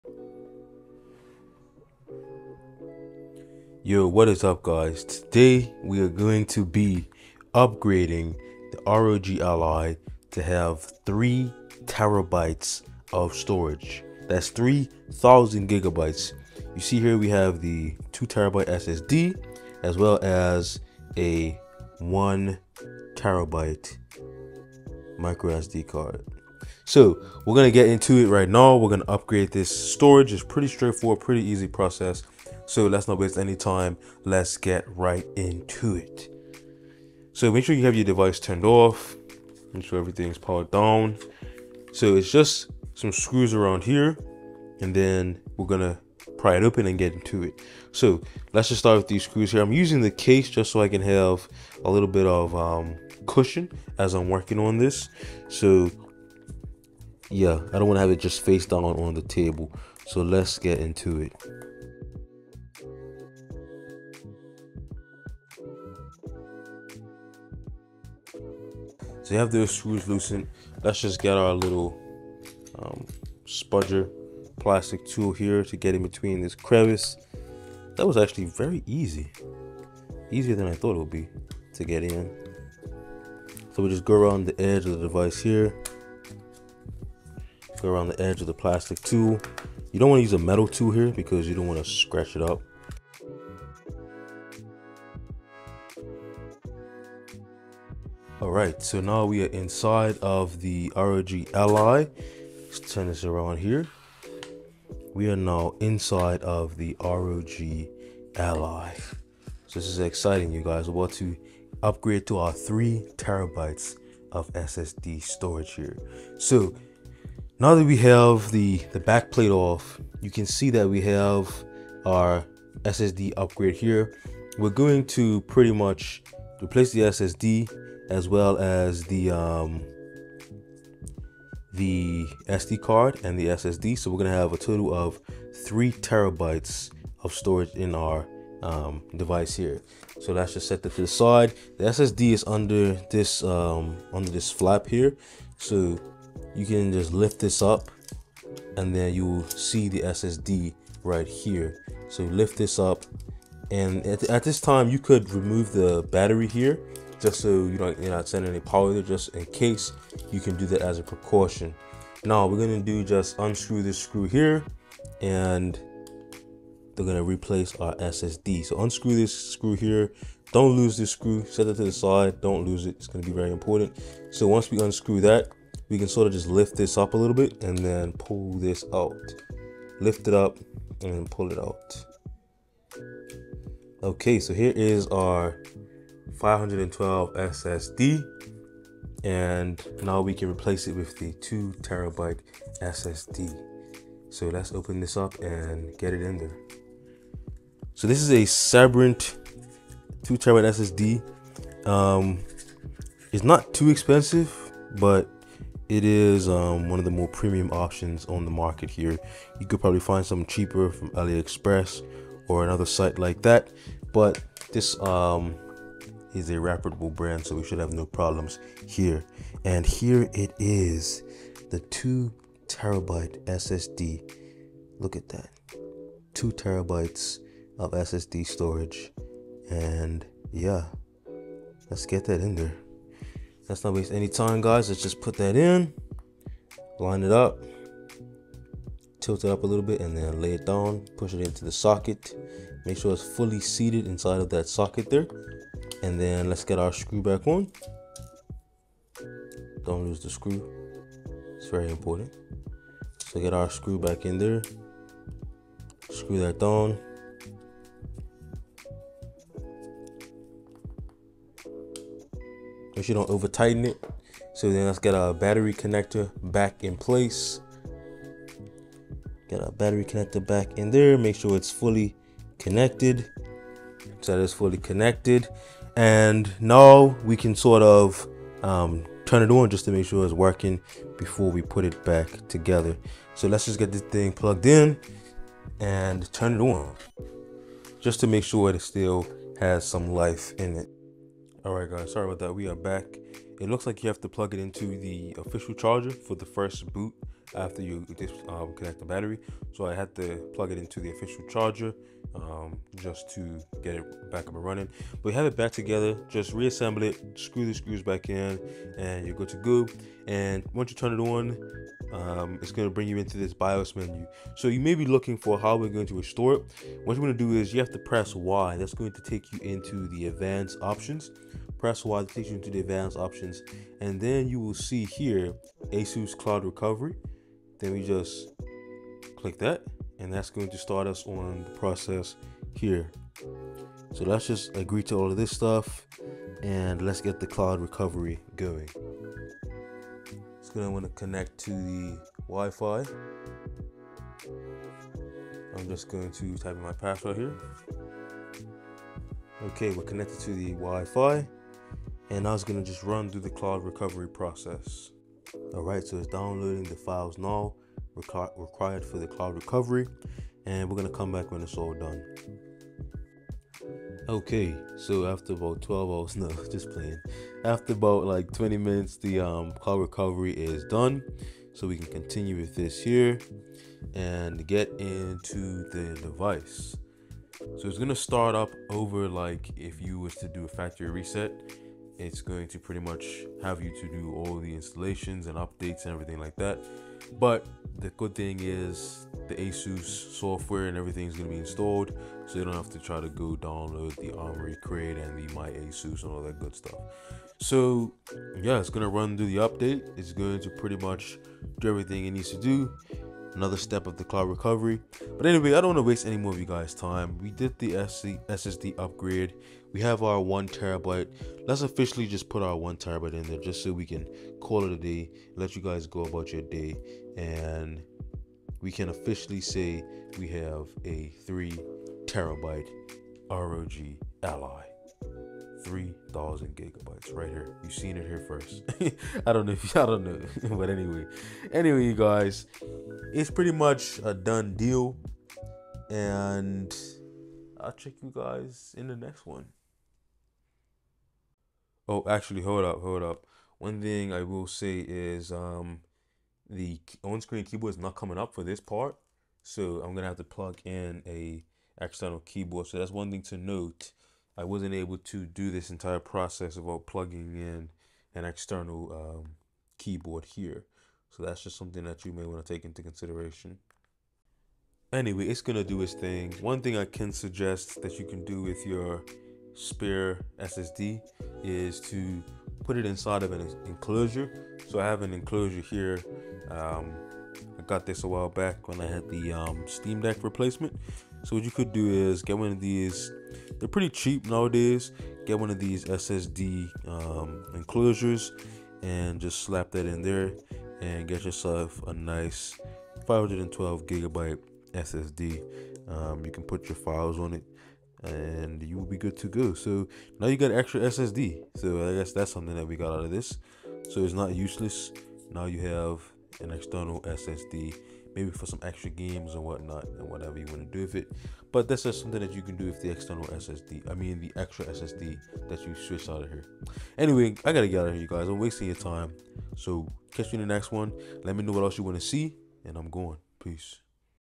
ROG Ally 2Tb SSD Replacement | 3Tb Upgrade!
It’s time to maximize the internal storage of the ROG Ally! We we are upgrading the SSD from 512 GB to a 2TB SSD. We will also be adding a 1Tb microSD card for a total of 3TB!